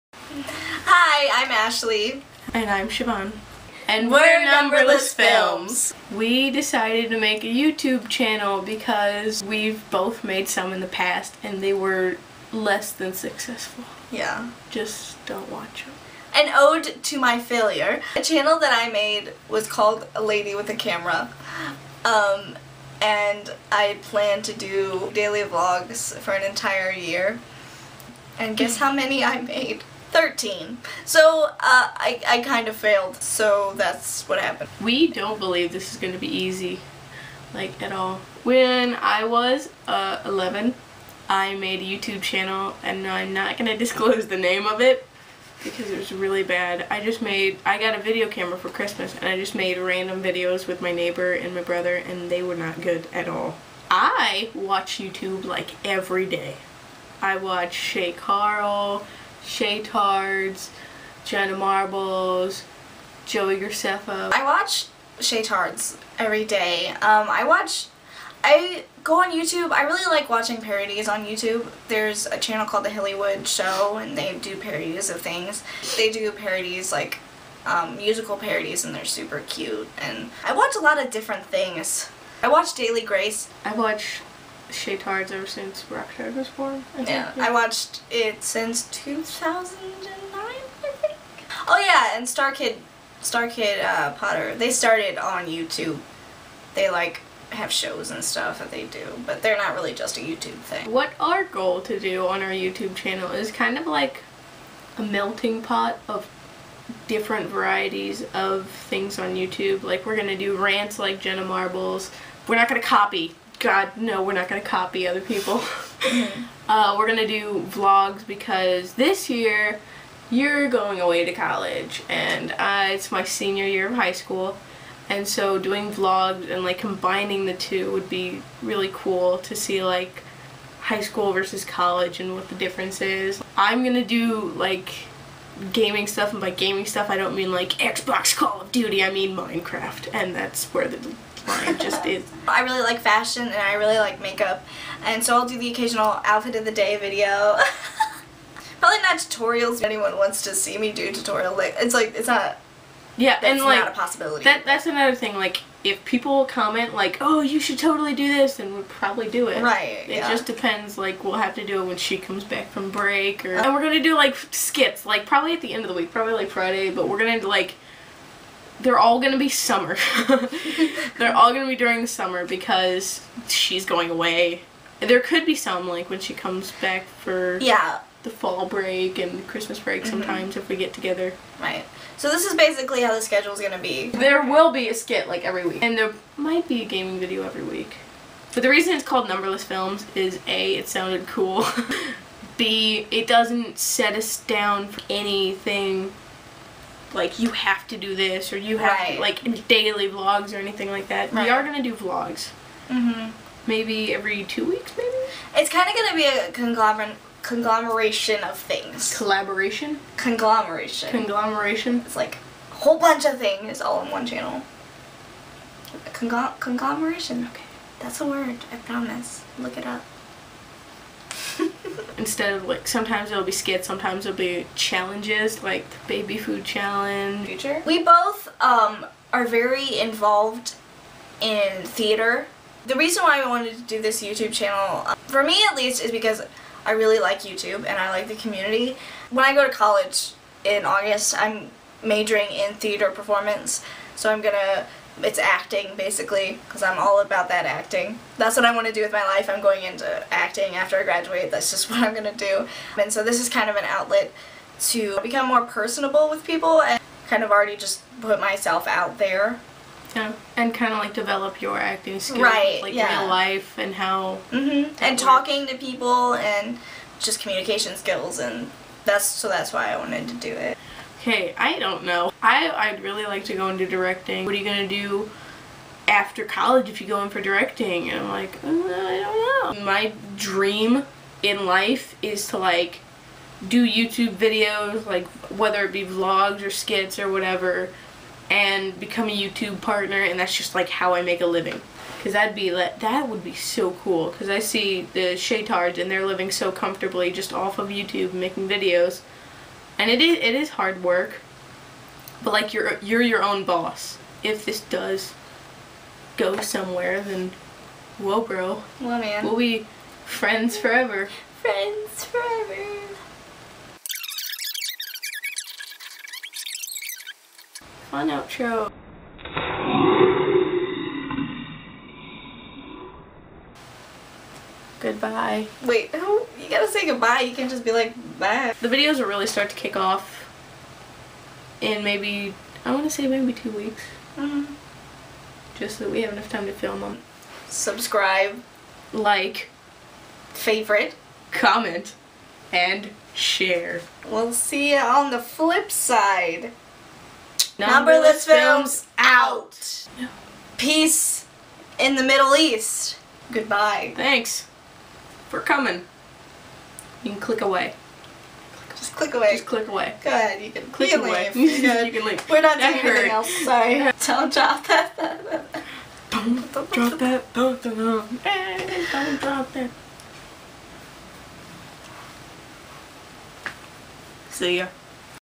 How We Use Youtube
***QUICK HEADS UP! It may be sideways, I'm sorry for that, it was difficult for me to flip it, however, if it is good and horizontal, I'm sorry it is so small, as time progresses I may get a better camera/ expierence with this so bare with me!***
    Hello and welcome to our journey into Youtube! We are very excited to make new videos and entertain you nice folk! We hope you enjoy our future videos, as this one is not all that funny, sorry. This video is more of an outlet to explain our goals on this channel, and to let you know what you'll be getting into. We aren't new to Youtube however we are new to this type of Youtube-ing
(psssst, we are also on Vine if you want to check that out as well!
Ashley Breanne and Siobhan I)
Filmed by: Bear
Edited by: Siobhan